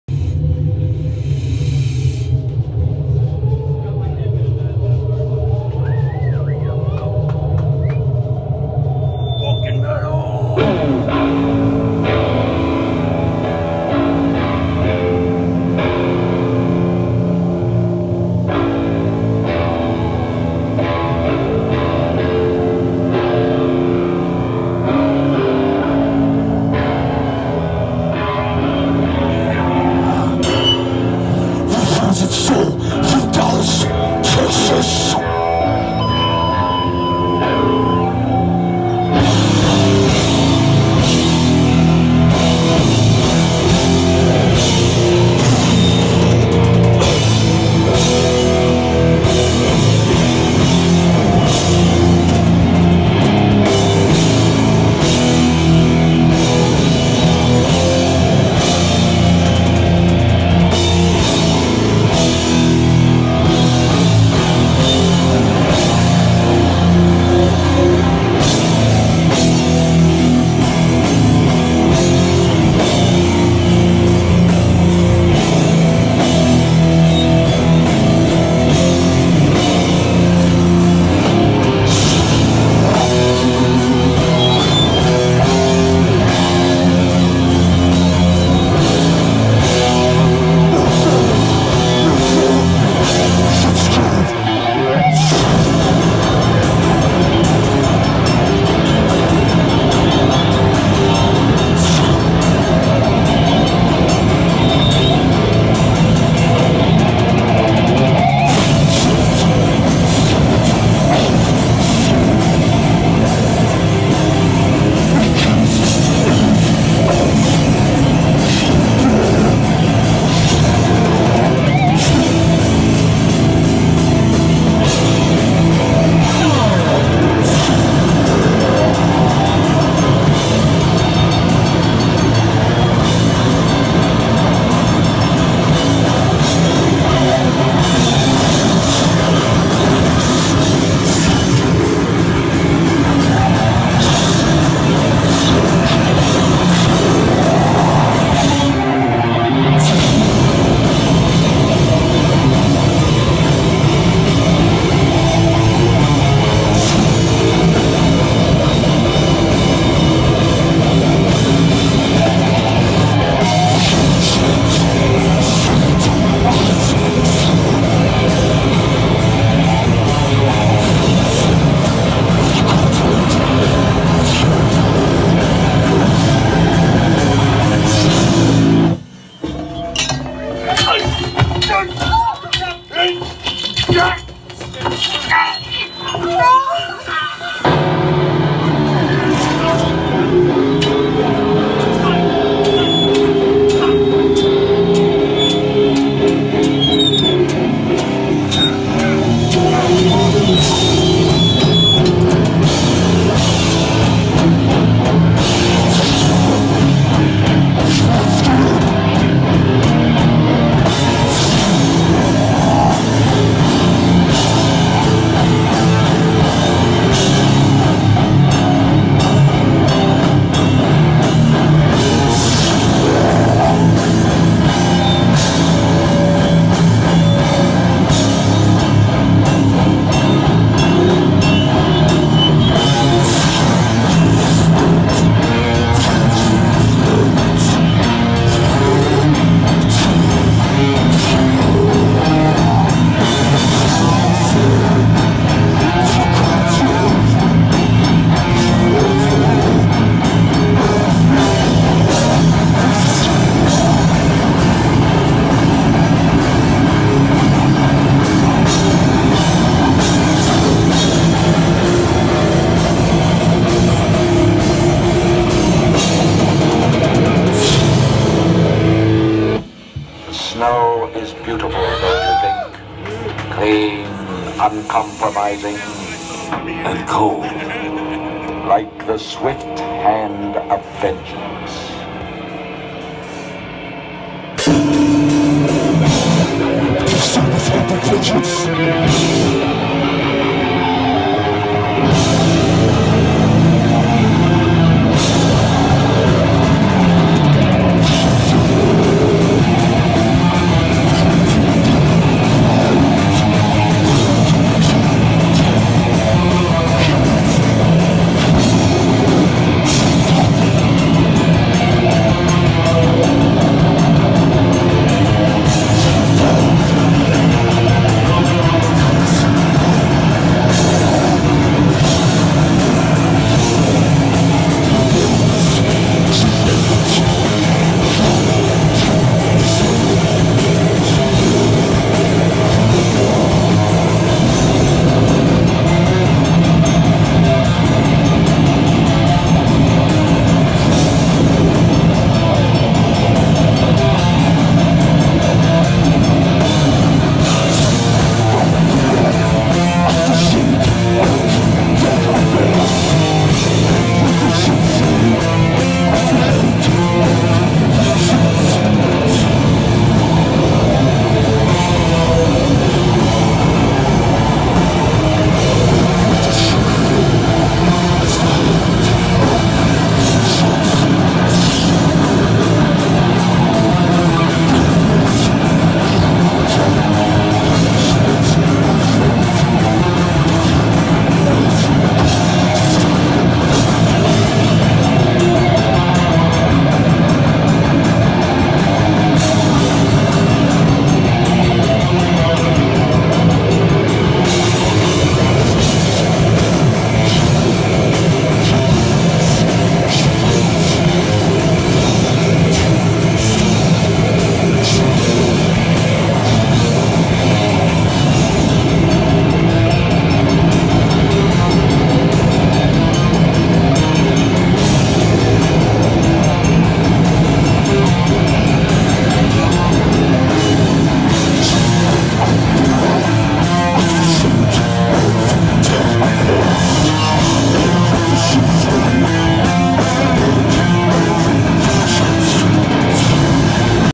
FROZEN SOUL-LIVE @THE NAIL: 01/04/2020
SORRY I RAN OUT OF STORAGE!

“Frozen Soul is an (no longer)obscure band (& Crushing Death Metal)from Texas and brings some great diversity into Maggot Stomp with their crushing sound. They use a good amount of echo on their vocals and instruments to make them sound like cave-dwelling cryptids; an effective mix that delivers on all brutal elements and creates a dense atmosphere.”
It’s HEAVY!!!